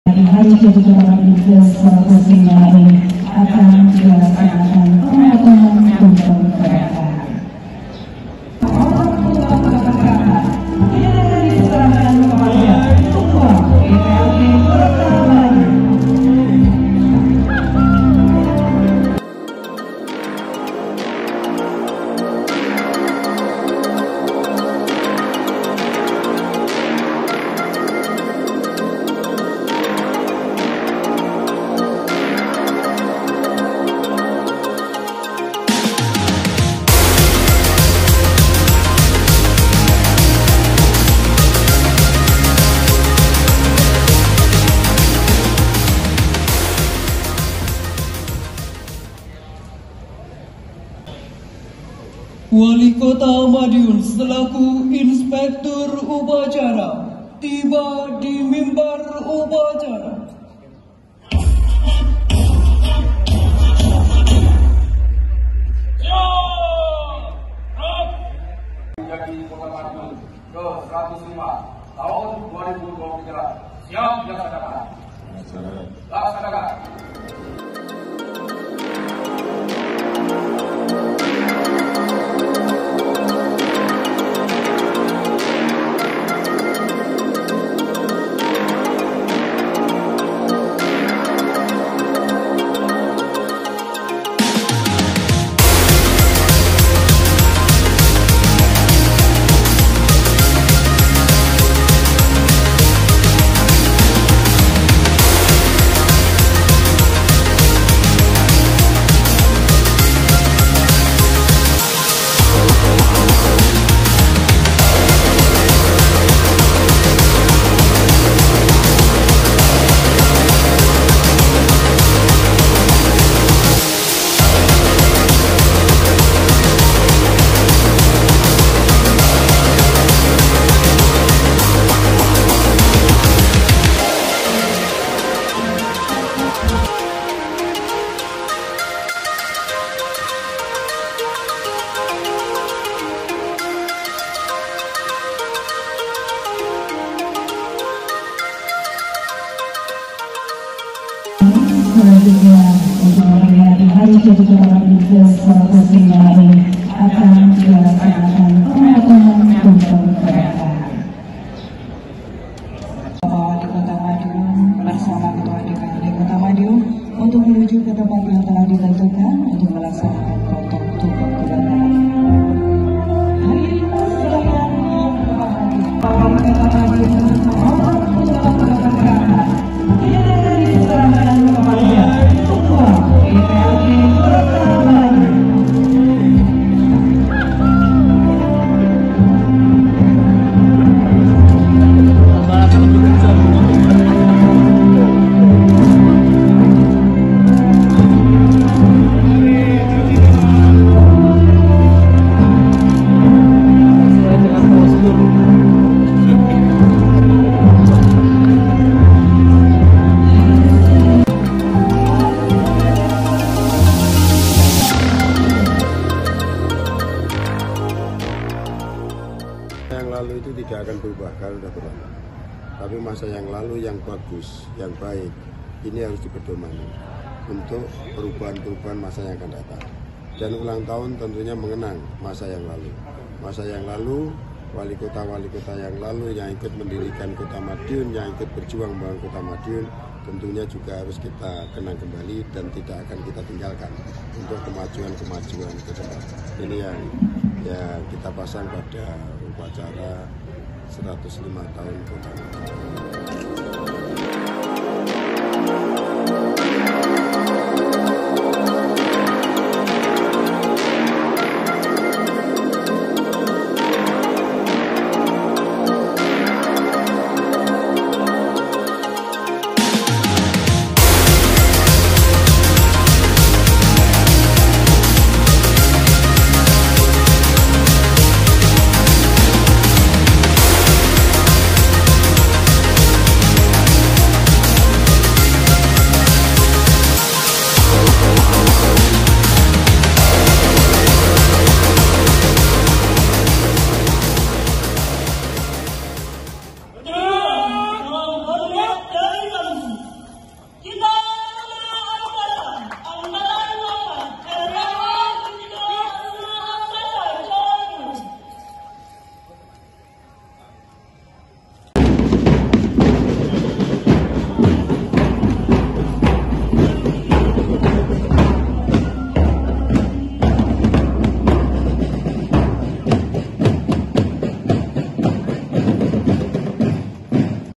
Aksi Pendekar Di Puncak HUT Kota Madiun ke 105
Upacara HUT Kota Madiun ke 105 Selasa 19 Juni 2023